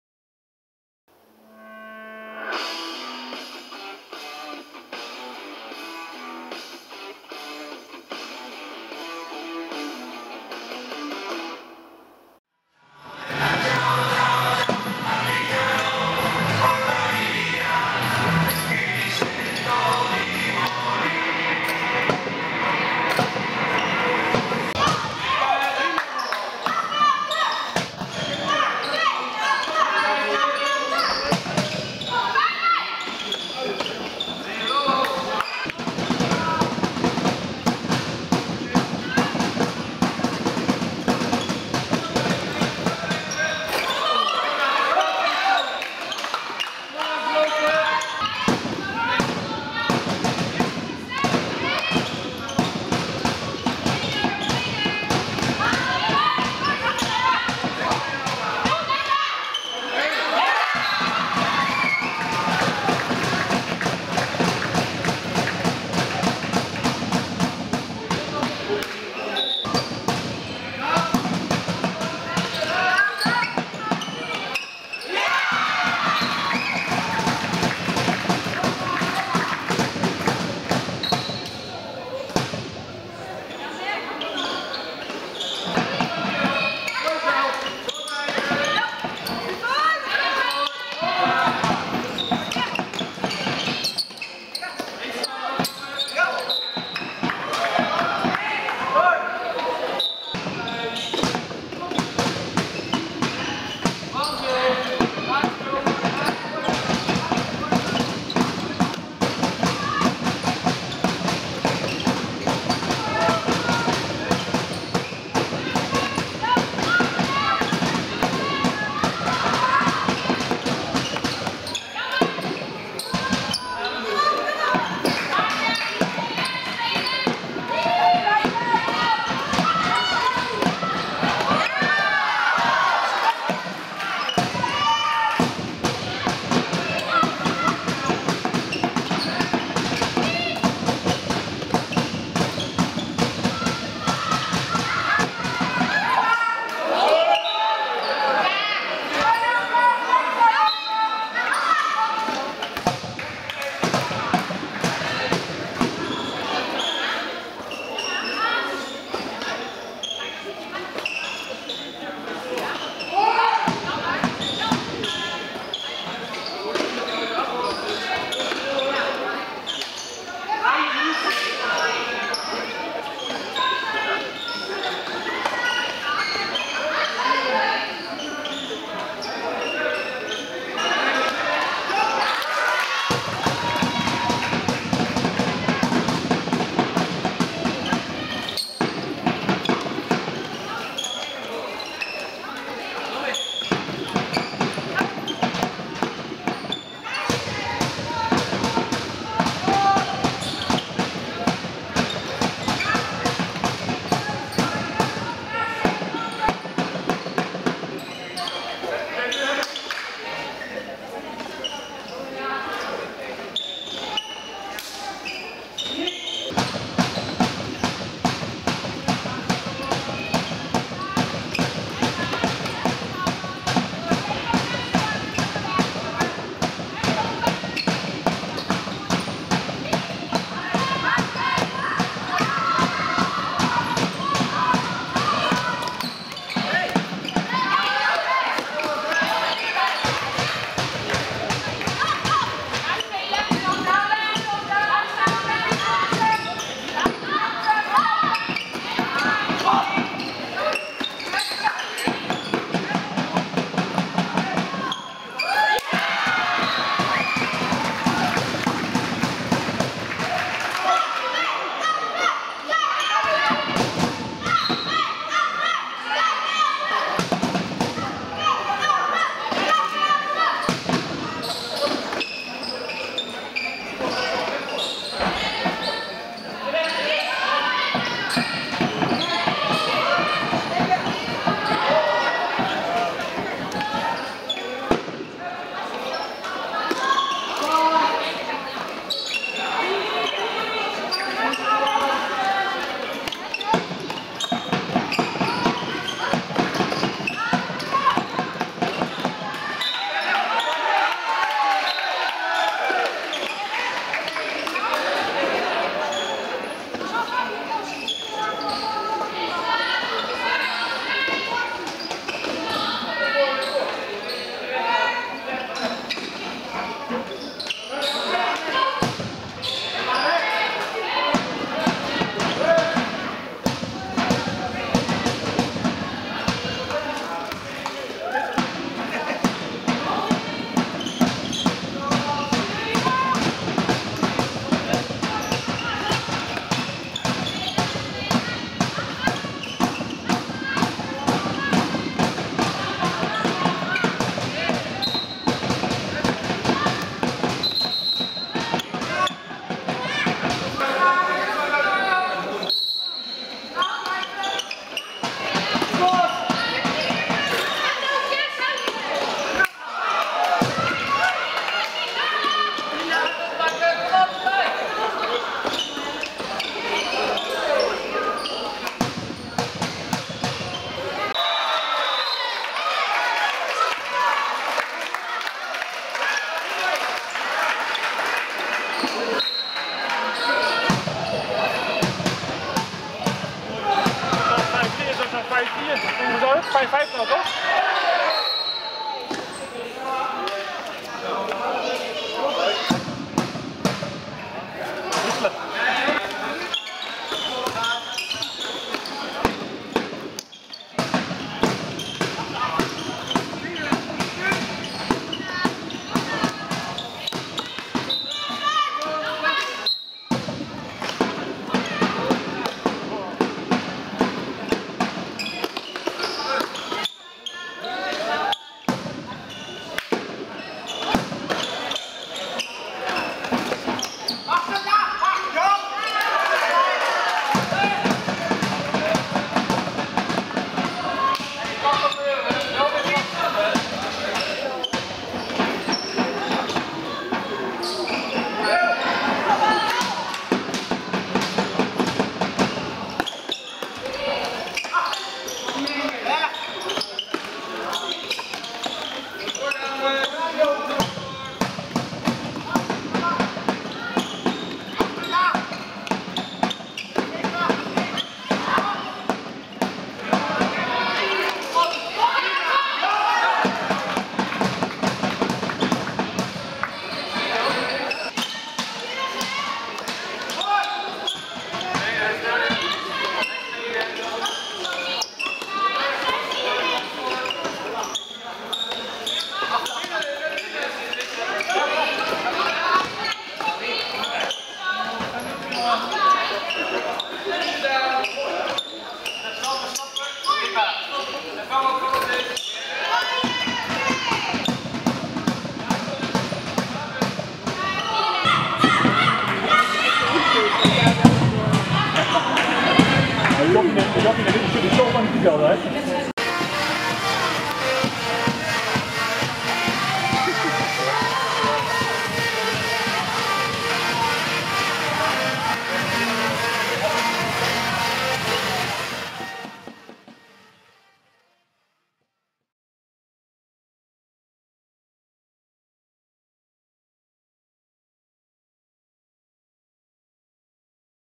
B1 kampioen? Korfbal zaal KVApeldoorn B1 - Juventa B1
korfball korfbal KVApeldoornB1 Kampioen! tegen Juventa B1

In Hardenberg haalde de B1 van Apeldoorn de kampioenschap binnen. Ze kregen de overwinning niet cadeau, maar hebben als team gewonnen.